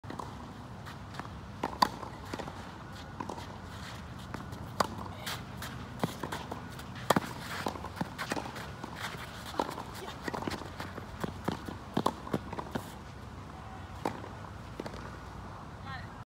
TWEENER by Tash
Natalya “Tash” Malenko hitting the TWEENER at The Cape.